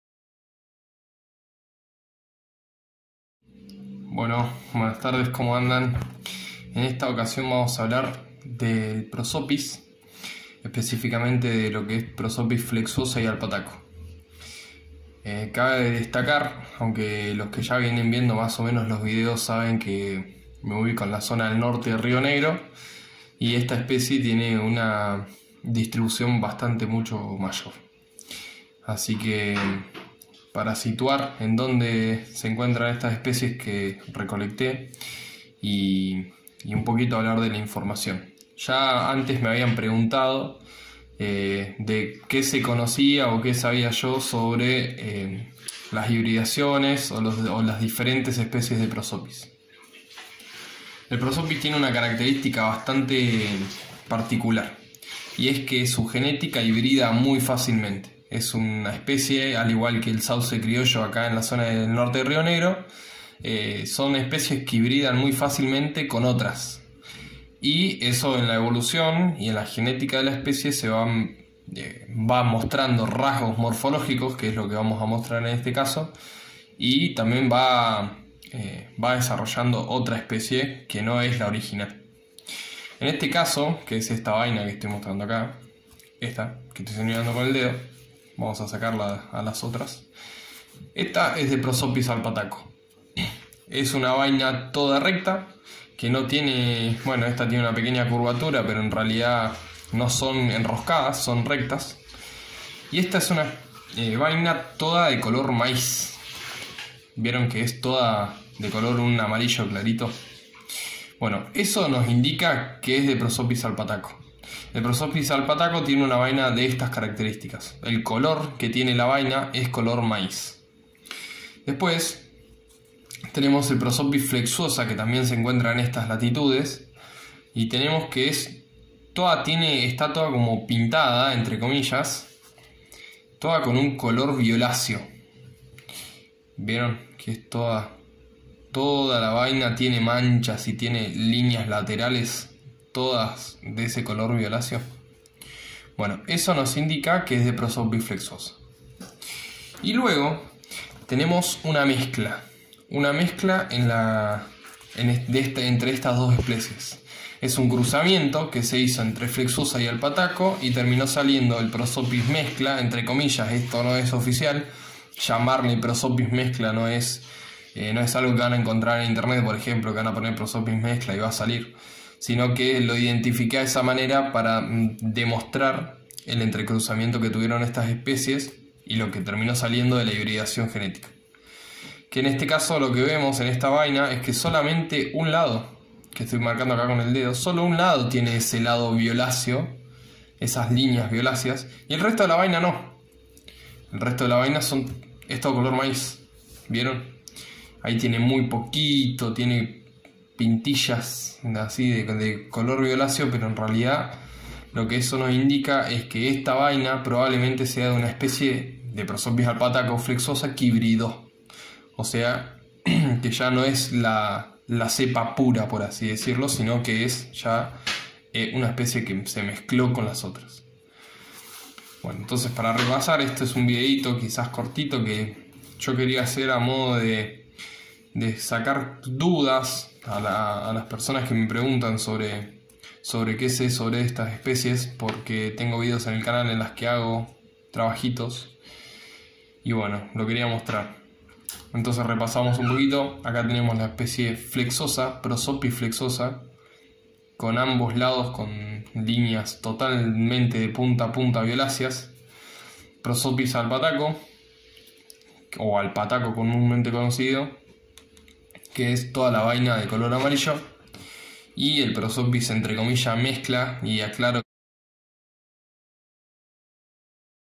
Diferencias entre Prosopis alpataco, flexuosa y "Mezcla".
En este video vamos hablar de la hibridación de la especie Prosopis alpataco y flexuosa en la zona al Norte de Rio Negro. Espero les guste. Saludos.